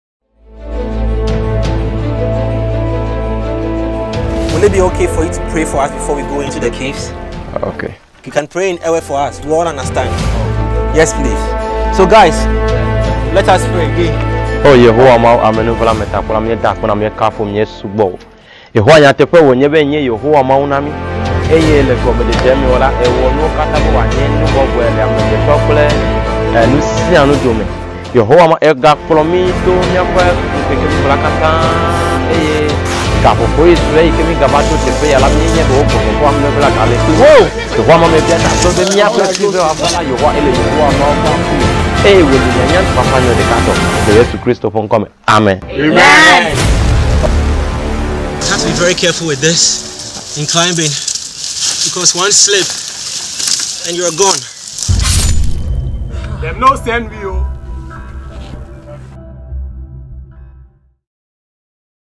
PRAY FOR US IN EWE | LIKPE TEDOME | ANCESTRAL CAVES | SKY MARSHALS GHANA
Join us experience Ancestral Caves in the volta region, discovered more than 200 years ago , Sky Marshals